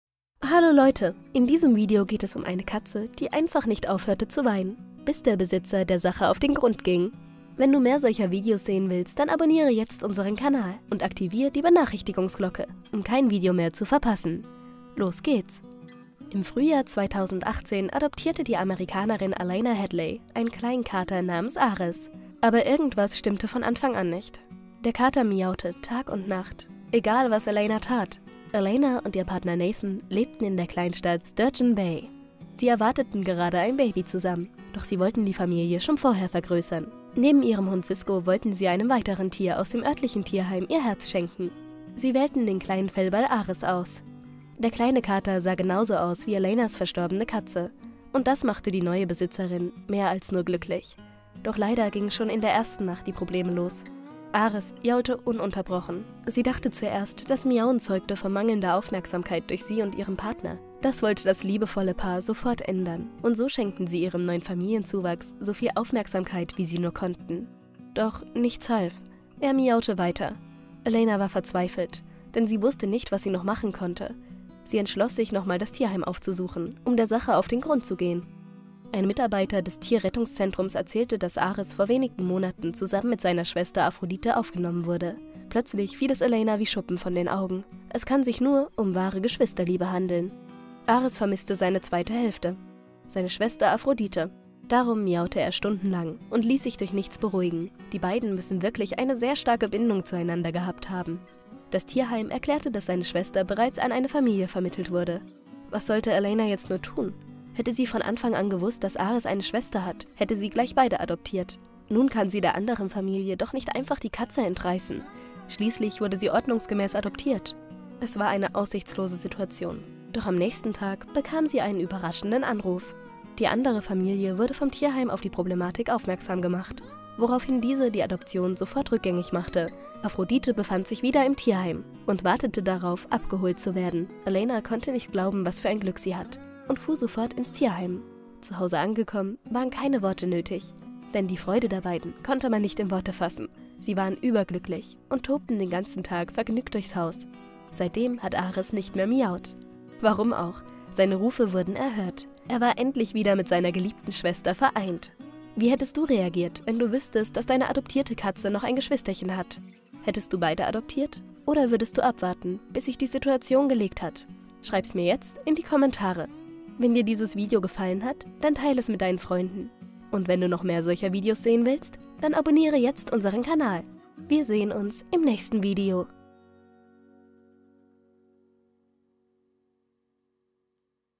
Kätzchen weinte jeden Tag, dann verstand die Besitzerin – Es will ihr etwas sagen
In diesem Video geht es um eine Katze, die einfach nicht aufhören kann zu weinen. Doch dann realisiert die Besitzerin warum...

Im Frühjahr 2018 adoptierte die Amerikanerin Alana Hadley einen kleinen Kater namens Ares. Aber irgendwas stimmte von Anfang an nicht. Der Kater miaute – Tag und Nacht -, egal was Alana tat.

Alana und ihr Partner Nathan lebten in der Kleinstadt Sturgeon Bay. Sie erwarteten gerade ein Baby zusammen, doch sie wollten die Familie schon vorher vergrößern. Neben ihrem Hund Sisko wollten sie einem weiterem Tier aus dem örtlichen Tierheim ihr Herz schenken. Sie wählten den kleinen Fellball Ares aus.

Der kleine Kater sah genauso aus wie Alanas verstorbene Katze und das machte die neue Besitzerin mehr als nur glücklich. Doch leider gingen schon in der ersten Nacht die Probleme los. Ares jaulte ununterbrochen.

Sie dachte zuerst, das Miauen zeugte von mangelnder Aufmerksamkeit durch sie und ihren Partner. Das wollte das liebevolle Paar sofort ändern und so schenkten sie ihrem neuen Familienzuwachs so viel Aufmerksamkeit, wie sie nur konnten. Doch nichts half. Er miaute weiter.

Alana war verzweifelt, denn sie wusste nicht, was sie noch machen konnte. Sie entschloss sich nochmal das Tierheim aufzusuchen, um der Sache auf den Grund zu gehen.

Was war das Geheimnis des kleinen Kätzchens? Warum weinte sie jeden Tag. Und wusste das Tierheim eine Antwort auf die Probleme der Katze? Das alles und noch einiges mehr erfährst du jetzt.

Von Generation Sonnenschein

[Copyright Hinweise Bilder]

[Rechtliches]